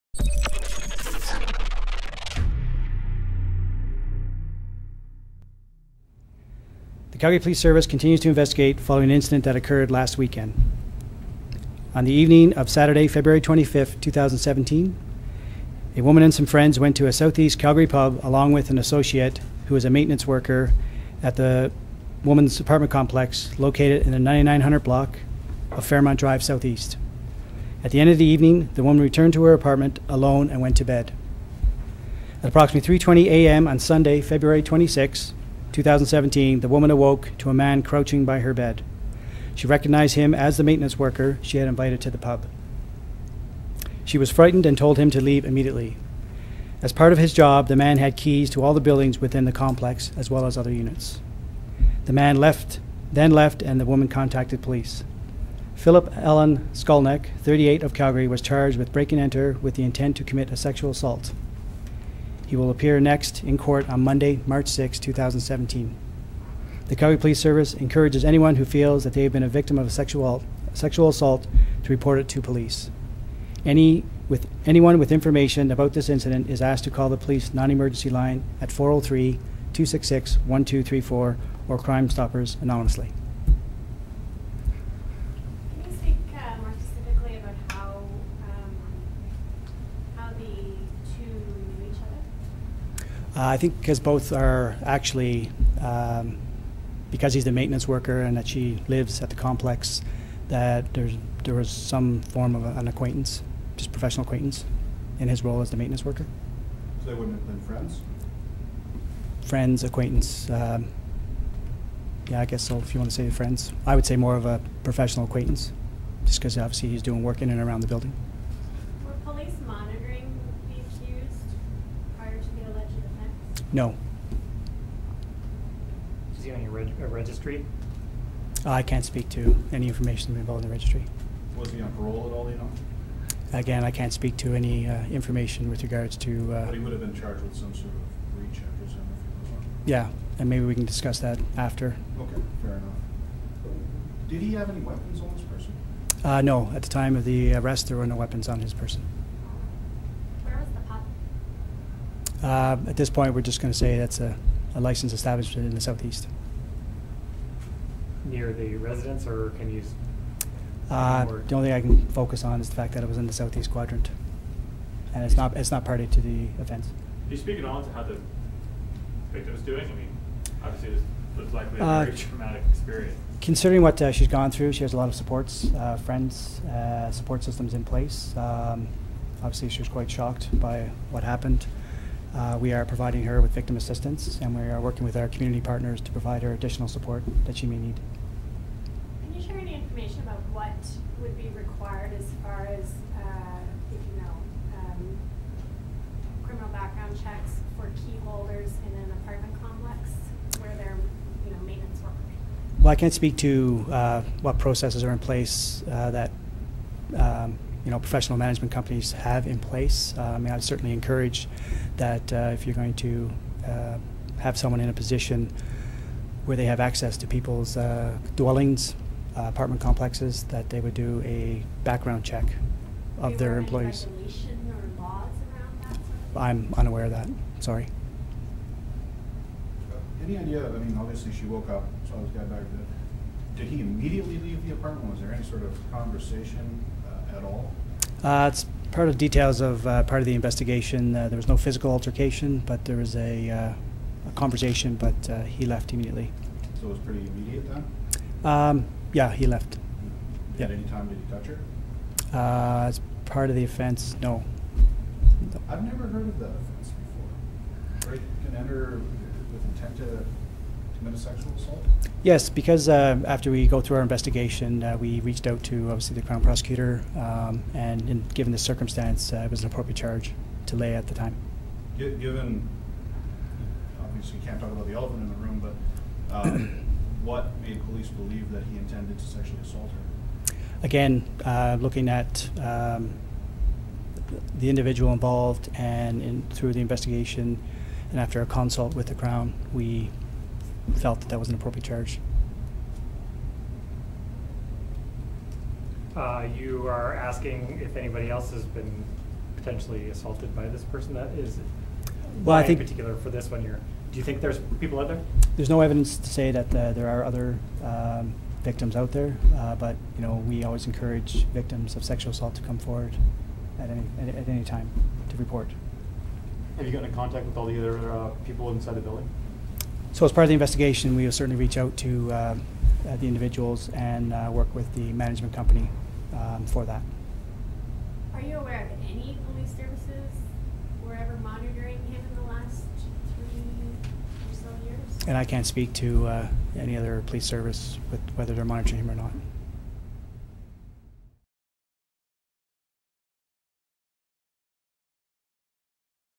Man charged with break and enter with intent to commit sexual assault
The Calgary Police Service continues to investigate following an incident that occurred last weekend.
On the evening of Friday, Feb. 25, 2017, a woman and some friends went to a southeast Calgary pub along with an associate who is a maintenance worker at the woman’s apartment complex.
At the end of the evening, the woman returned to her apartment alone and went to bed.
At approximately 3:20 a.m., on Saturday, Feb. 26, 2017, the woman awoke to a man crouching by her bed. She recognized him as the maintenance worker she had invited to the pub. She was frightened and told him to leave immediately. As part of his job, the man had keys to all buildings within the complex, as well as all units. The man then left and the woman contacted police.
Philip Allan SKULNEC, 38, of Calgary, was charged with break and enter with intent to commit a sexual assault. He will next appear in court on Monday, March 6, 2017.
The Calgary Police Service encourages anyone who feels they have been a victim of sexual assault to report it to police.
Anyone with information about this incident is asked to call the police non-emergency line at , or Crime Stoppers anonymously using any of the following methods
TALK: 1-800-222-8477
TEXT: tttTIPS to 274637
Case CA17079997/4012